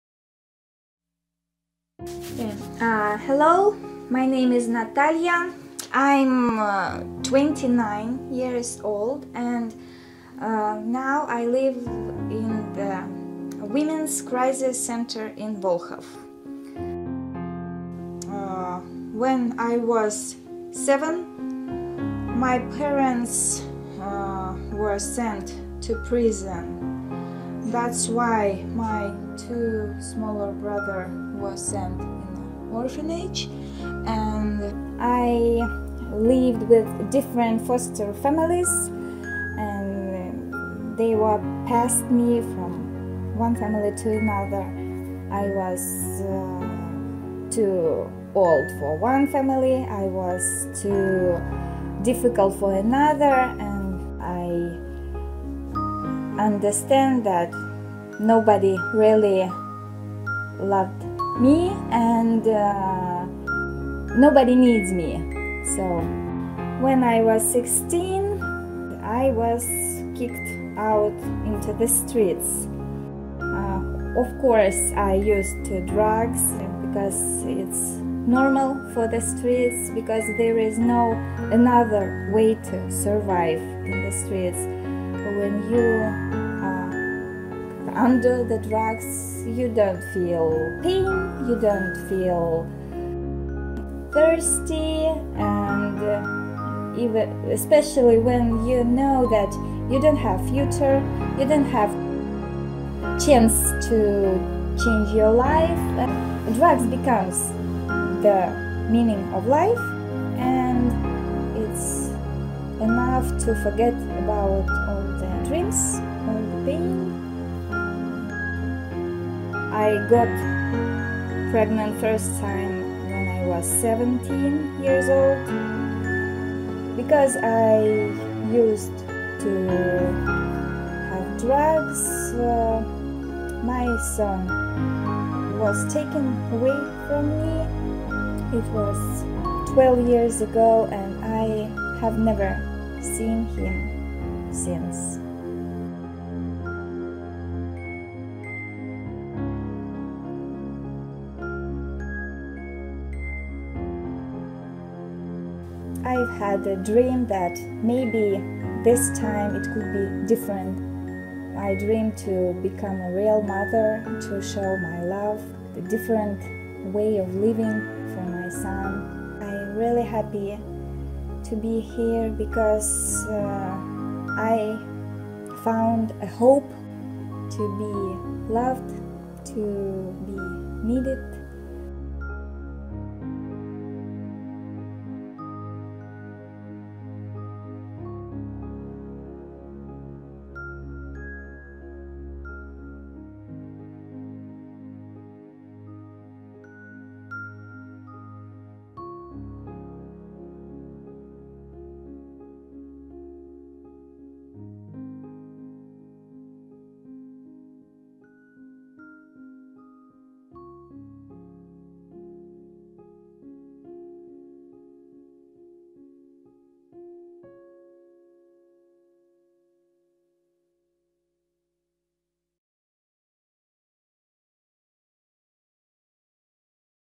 The Moses Appeal
This is Natalia's* Story about how she came to be needing the Moses Project and the women's crisis centre.
* Name changed to protect privacy.